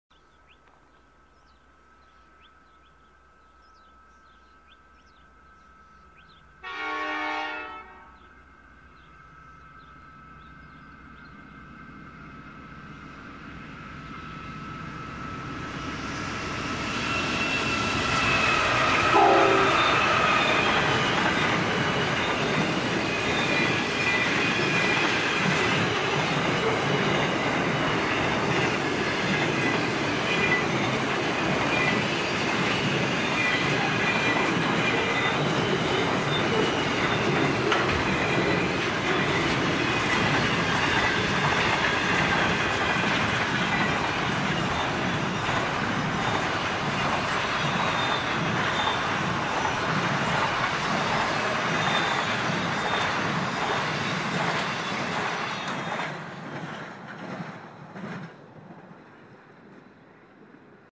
4PM4 Steel & Container Wagons - PoathTV Australian Trains & Railways
Pacific National Perth to Melbourne train conveying steel products in open wagons and covered containers, included long pieces of rail spanning pairs of wagons. Containers of general freight are also included in the consist.

Locomotives:  NR72 + NR98,  41 wagons